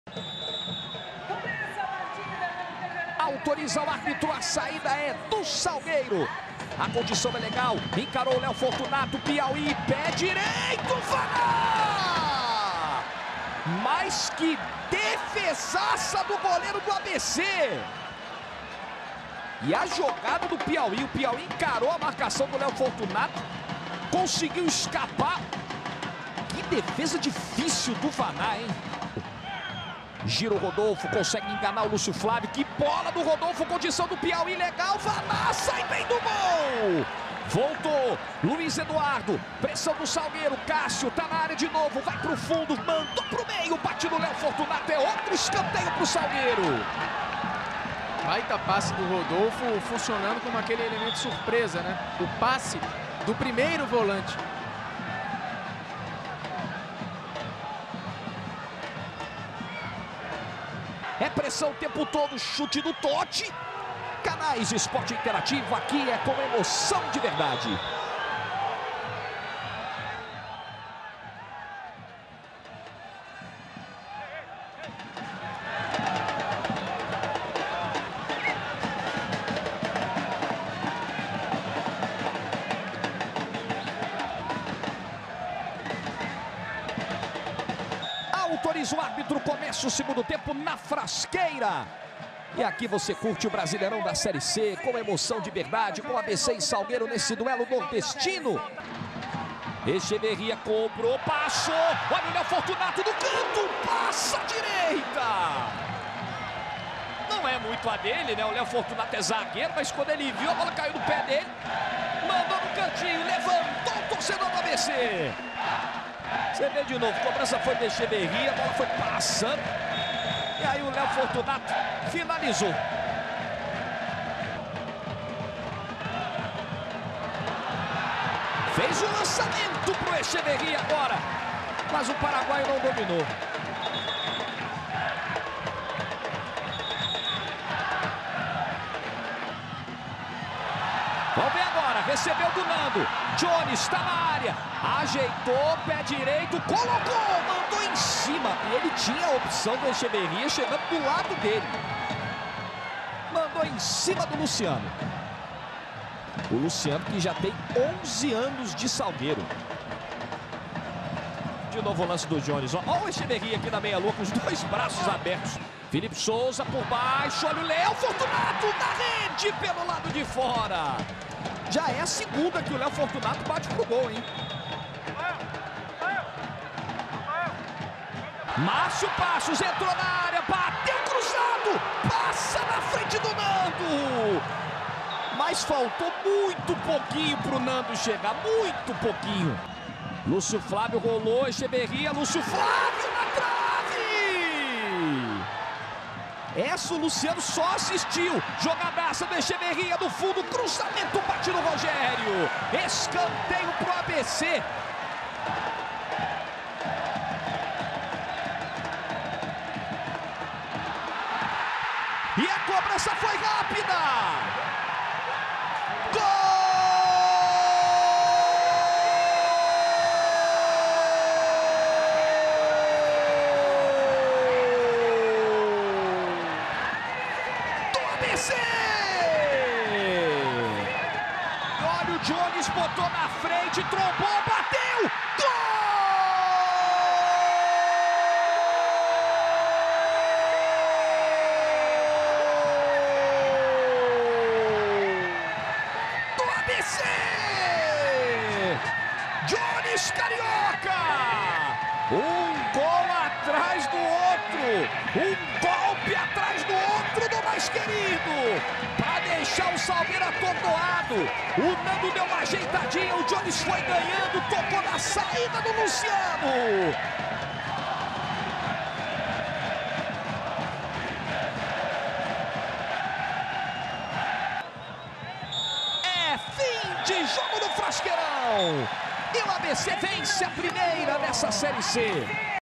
Série C 2016: 2ª rodada – ABC 2 x 0 Salgueiro/PE
Campeonato Brasileiro Série C 2016 – ABC 2 x 0 Salgueiro/PE

– Data da partida: 28/05/2016
– Frasqueirão – Natal (RN).
– 2ª Rodada
– 1ª Fase

Márcio Passos e Jones Carioca marcaram os gols da vitória do Mais Querido.

Imagens: Esporte Interativo